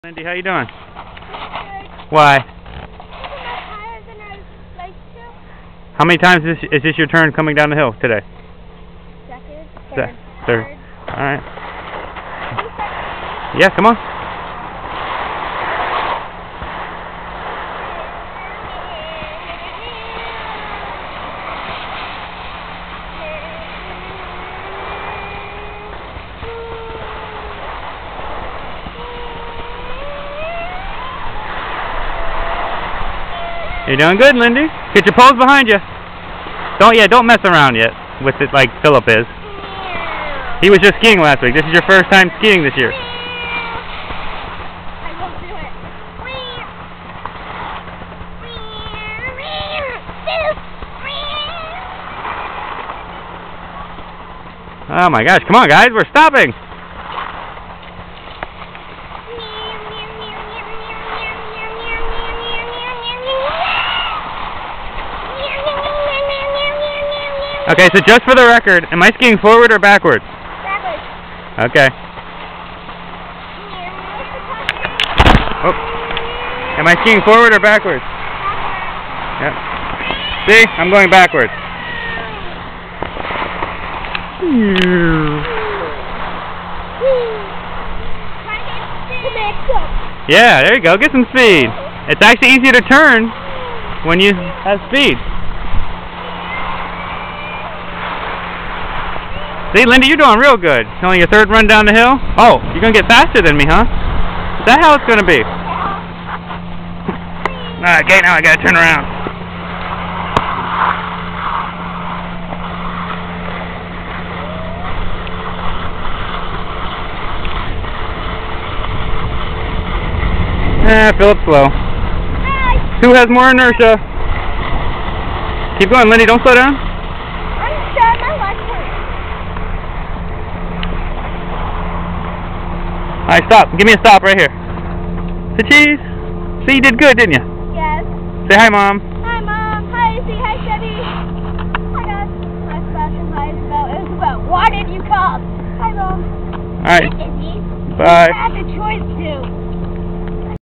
Spring Mountain with Phillip and Lindy March 2012
Lindy's first time skiing.  Spring Mountain near us has a great FREE 4th grade ski program.  So Phillip is in 4th grade in this video.